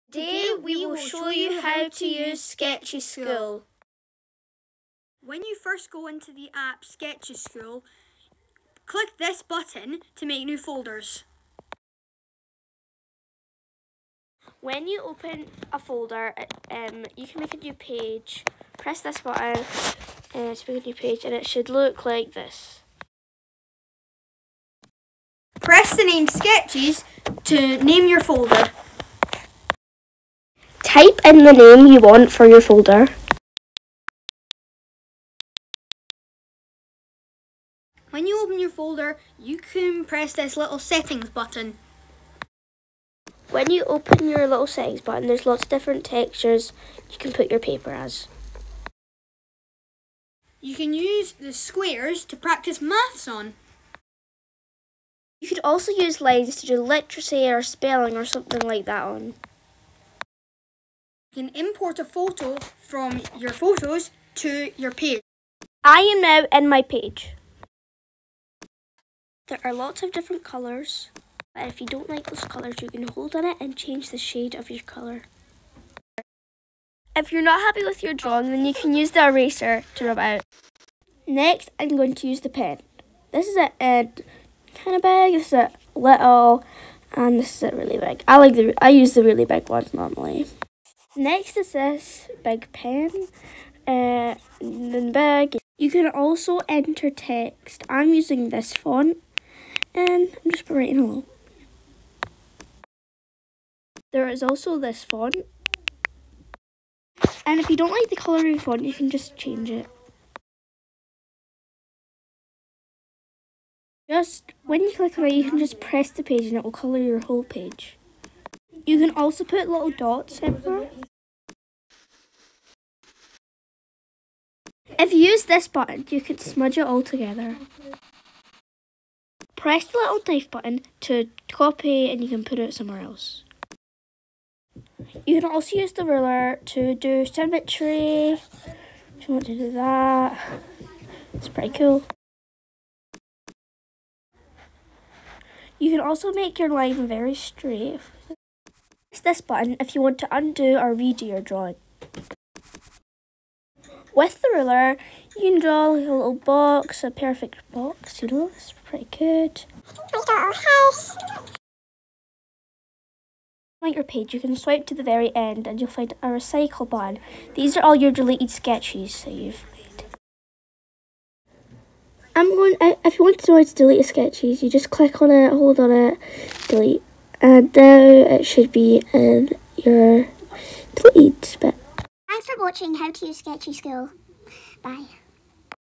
SKETCHESSCHOOL How to Guide
Step by step guide to using SKETCHESSCHOOL by Digital Leaders